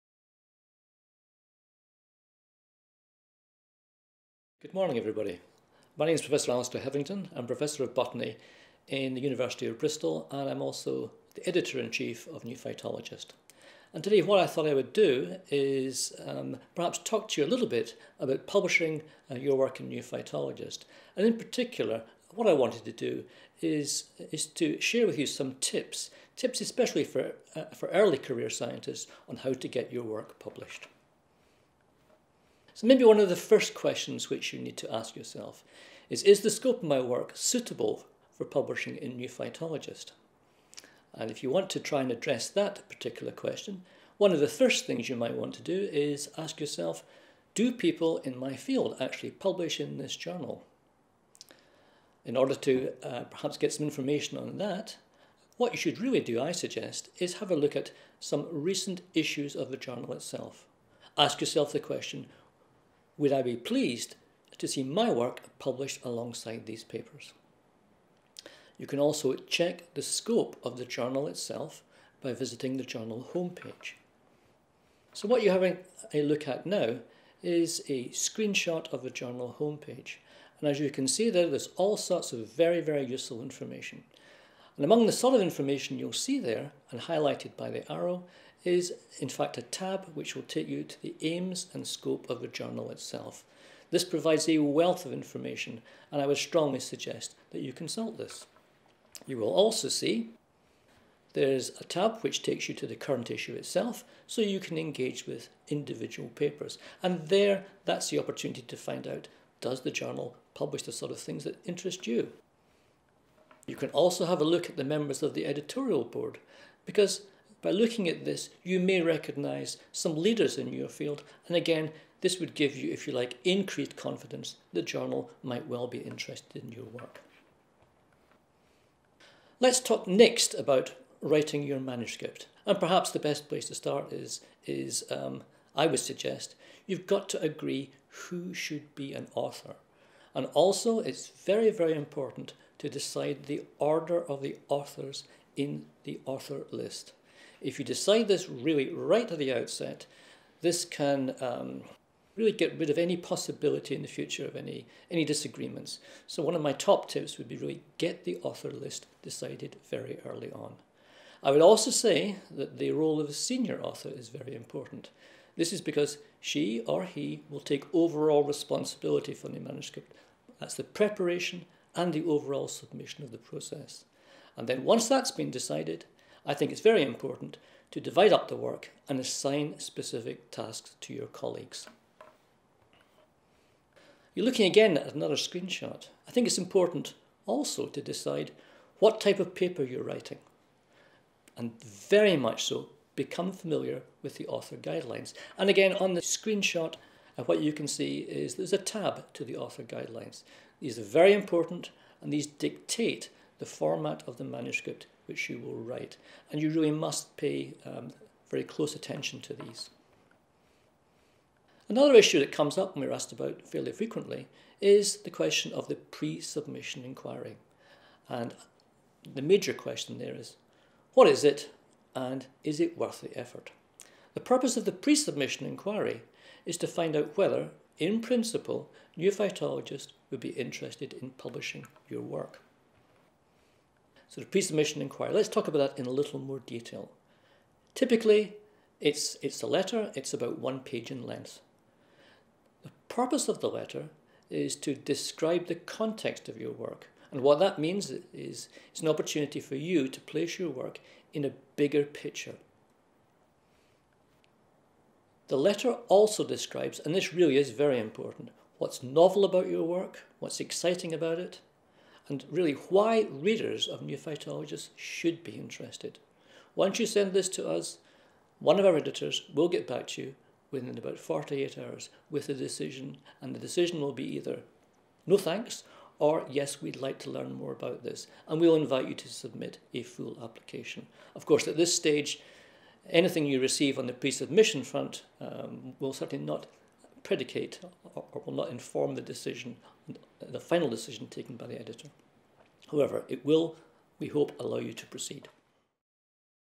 Publishing in New Phytologist - part one: Should I submit my work to New Phytologist?
How do you decide whether to publish your work in New Phytologist? Editor-in-Chief Prof Alistair Hetherington shares his top tips for early career researchers in this short video.